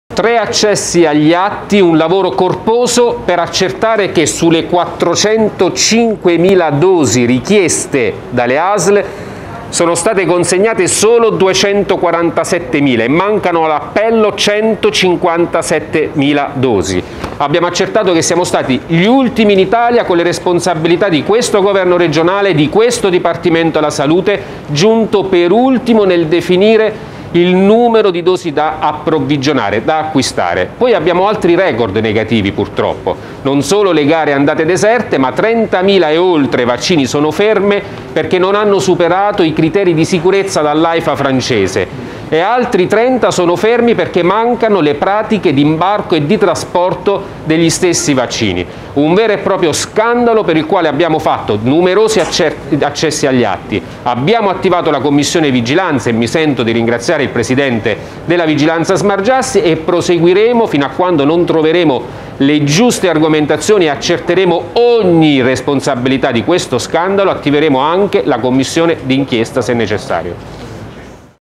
Il centrosinistra sui vaccini anti influenzali in Abruzzo
Le dichiarazioni del Capogruppo del Pd Silvio Paolucci a margine della conferenza stampa alla quale hanno partecipato i Consiglieri del centrosinistra Antonio Blasioli e Sandro Mariani